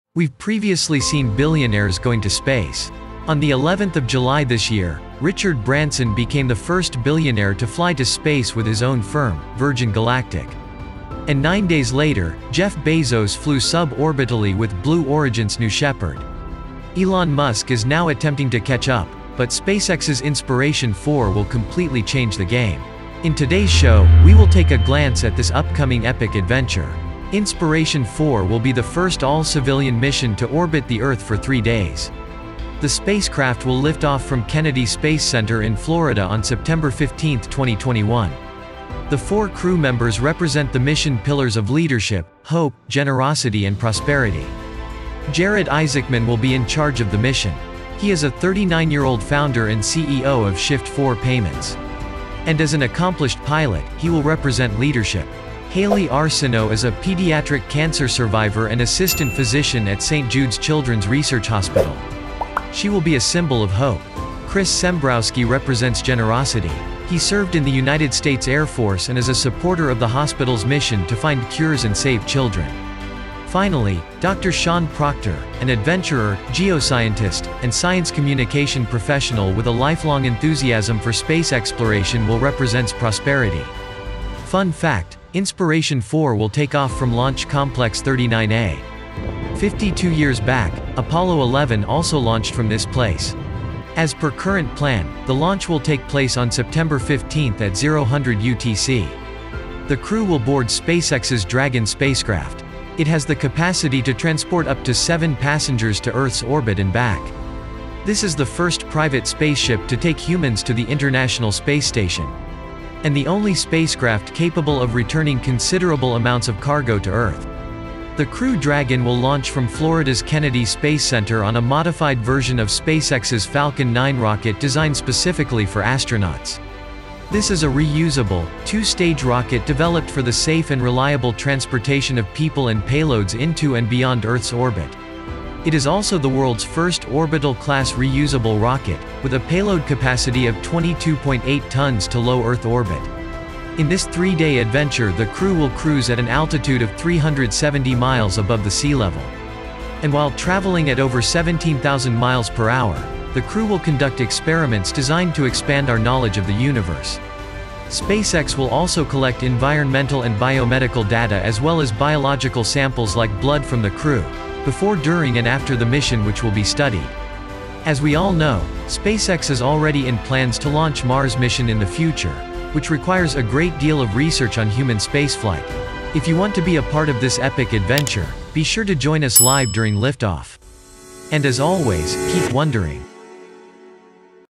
SpaceX Inspiration 4 | First Civilian SpaceFlight To Orbit
We've previously seen billionaires going to space. On the 11th of July this year, Richard Branson became the first billionaire to fly to space.
And nine days later, Jeff Bezos flew sub orbitally with Blue Origin's New Shepard.
Elon Musk is now attempting to catch up, but SpaceX’s Inspiration 4 will completely change the game.
In today's show, we will take a glance at this upcoming launch of "All-Civilian Mission To Orbit".

Timestamps
00:00 Intro
00:27 Inspiration 4
00:38 Crew Members
01:24 Launch
01:39 The Mission
02:47 Conclusion